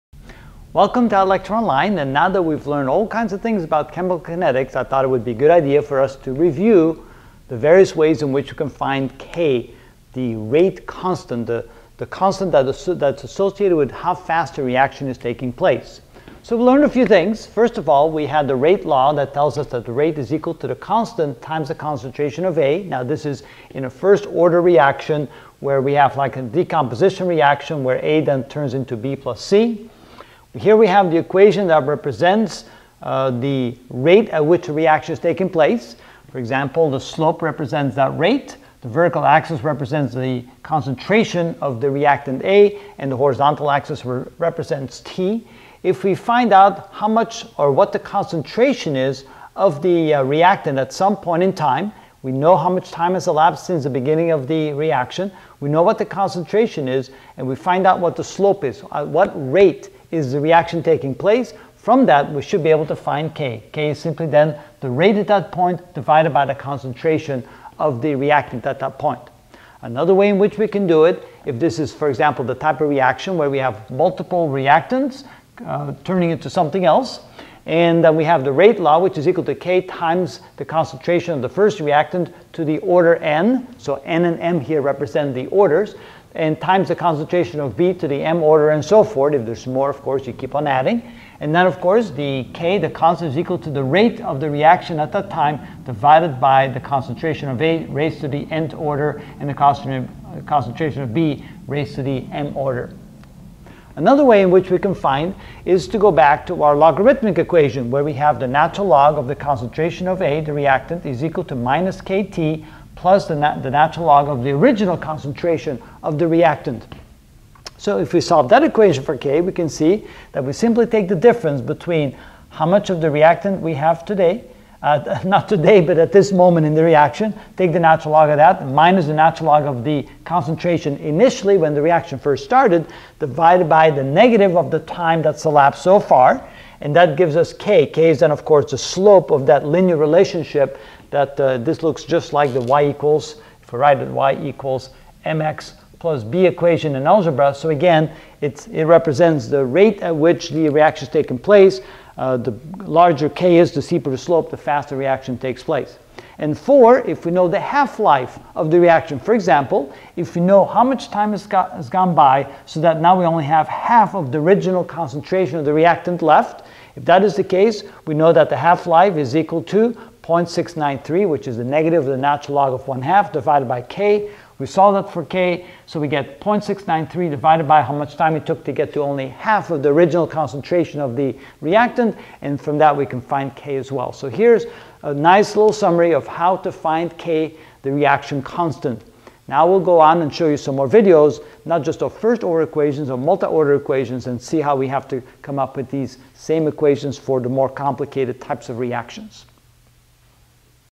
Chemistry - Chemical Kinetics (19 of 30) How to Determine the Rate Constant - Summery
In this video I will give a summary of determining the rate constant.